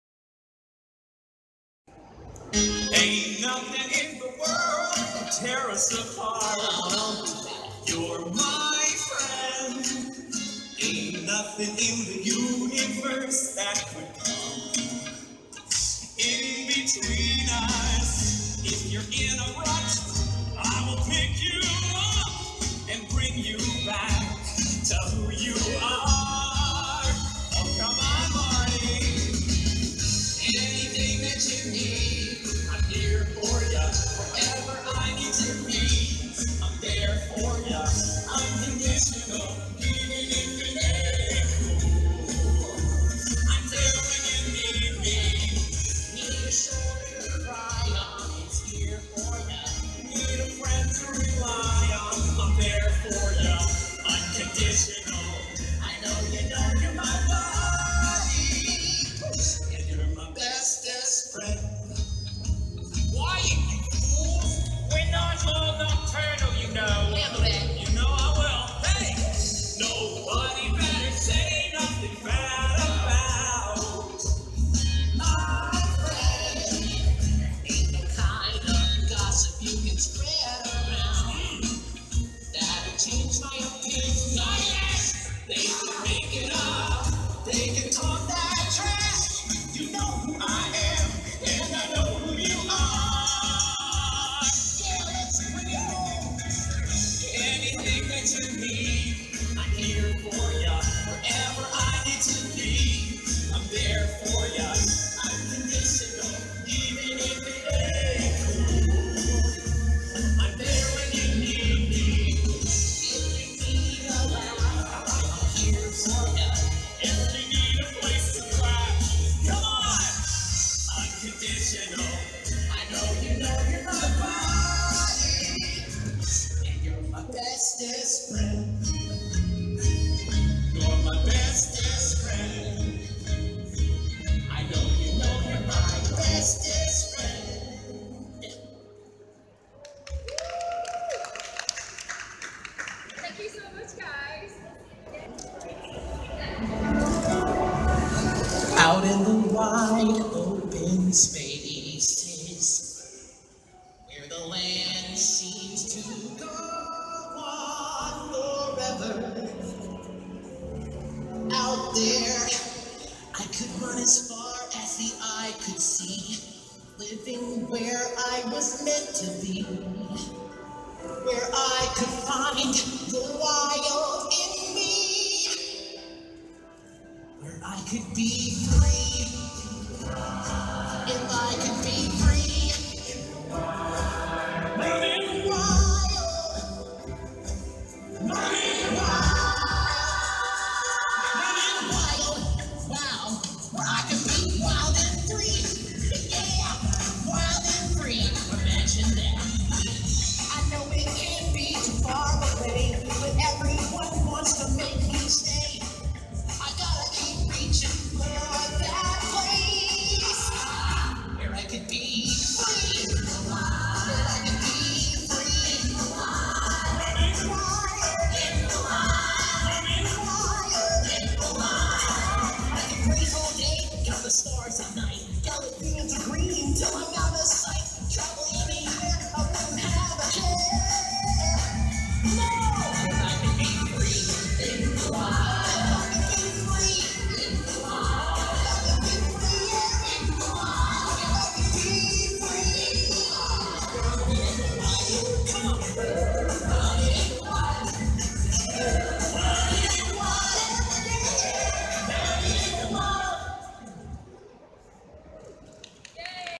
Madagascar Live! Preview
Madagascar Live! is starting in Singapore soon and here's a preview of the show.